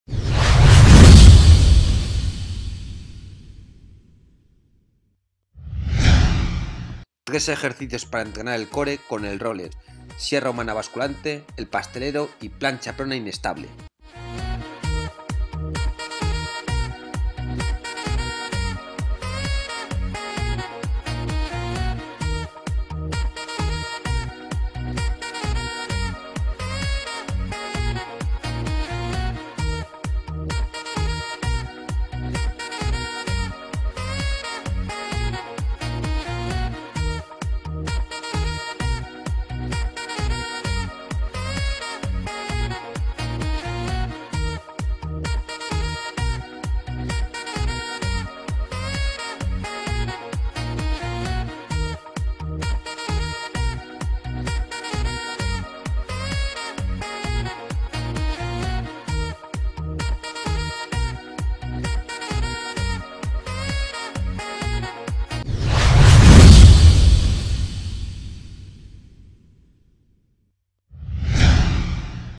Entrenamiento de Core (Abdominales): 3 variantes con Roller
Entrenamiento de Core (Abdominales): 3 variantes con Roller desde plancha frontal

1) Sierra humana basculante

Mezcla de 2 movimientos, sierra humana y flexión de cadera.

Adelantar y retrasar los hombros desde plancha prona, con una ligera basculación final de la pelvis, generada en la articulación coxofemoral (primera parte del movimiento de elevación de cadera prona).

2) El Pastelero:

Desde plancha prona pies y manos, hacer rodar el roller hacia delante y hacia atrás, con ambas manos de forma alternativa y consecutiva.

Mantener el equilibrio y la estabilidad mediante la activación de todo el core, implicando tanto la musculatura superficial abdominal (recto anterior del abdomen y oblicuos externos), como la más profunda (oblicuos internos y transverso abdominal).

3) Plancha prona inestable sobre roller:

Mantener la plancha prona clásica en 4 apoyos, pero apoyando ambas manos encima del roller, lo que nos genera cierta inestabilidad, al aumentar la activación muscular por una mayor coordinación intramuscular.

- 20% siguientes compras: JOSEMI20

Deportista y Editor: Josemi del Castillo
CCAFYDE Colegiado 55.255
Entrenador Personal Madrid
Vocal COPLEF Madrid